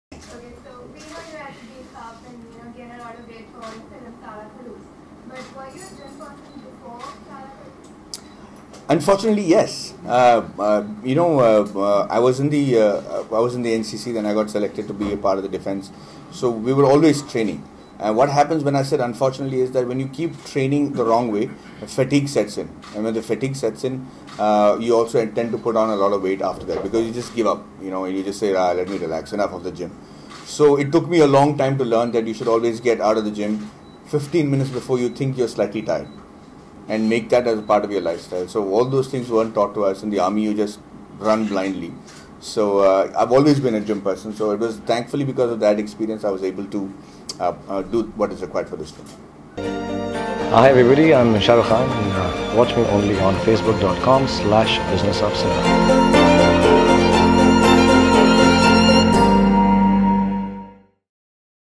R Madhavan Talks About Physical Training For Saala Khadoos!
Bollywood actor R Madhavan is featured on a Health magazine. Yesterday, the actor unveiled the magazine cover and talked about the being featured on it. The actor spoke about his past work that helped him in the film.